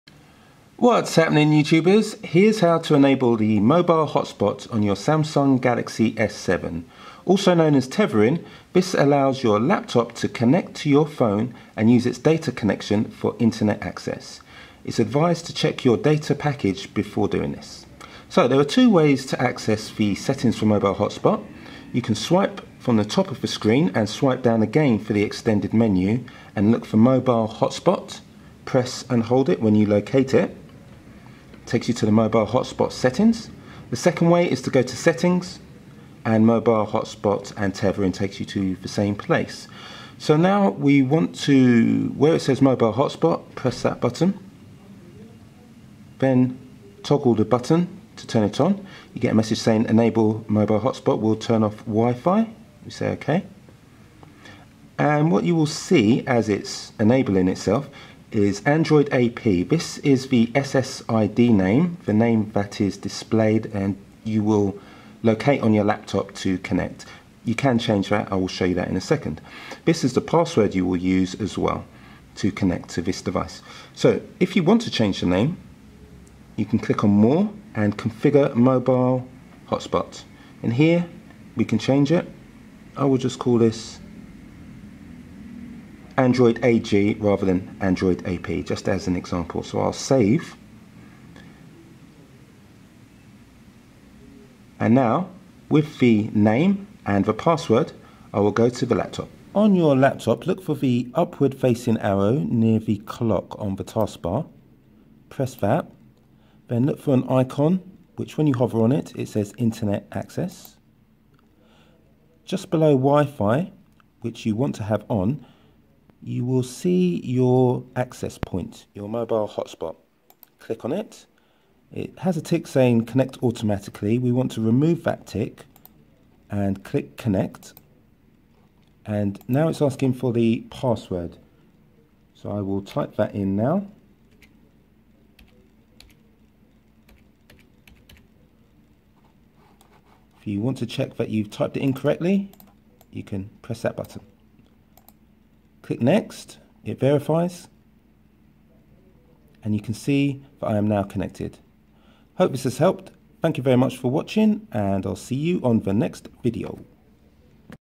S7: How to Setup Tethering - Mobile Hotspot - Samsung Galaxy S7 / Edge
Here's how to enable tethering for the Samsung Galaxy S7 / Edge Android phone. Also shows connection from the laptop.

Galaxy S7 Extended Battery Case

Samsung Wireless Charger

Samsung Galaxy Buds